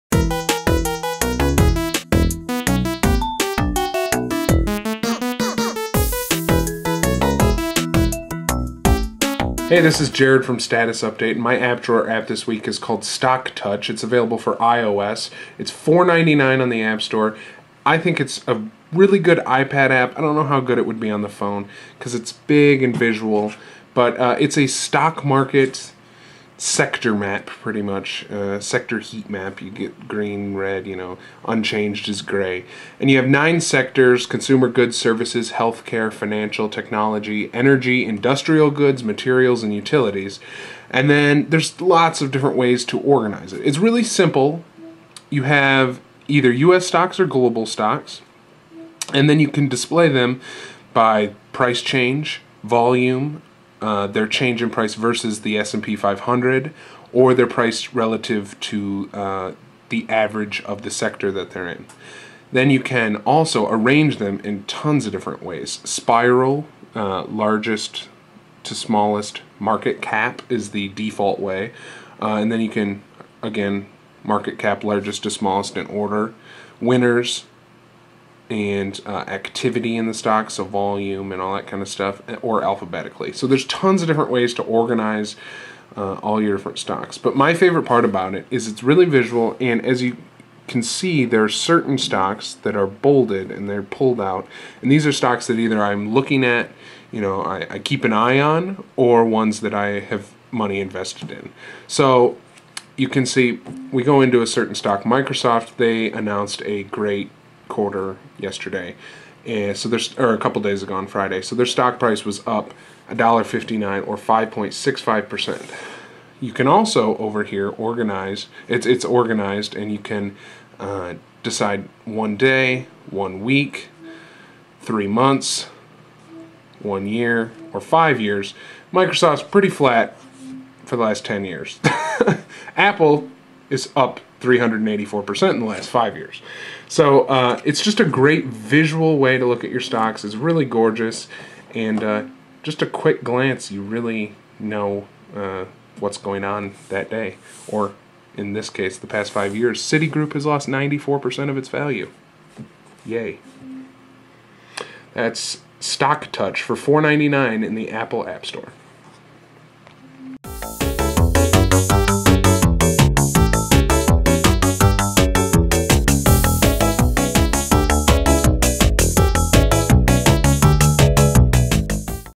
App Drawer - StockTouch
Jared takes a look at Stock app StockTouch for the iPad and iPhone. StockTouch provides a fantastic interface and an unique way to keep up-to-date on all your stocks.

Status Update is a weekly social media and social tech podcast.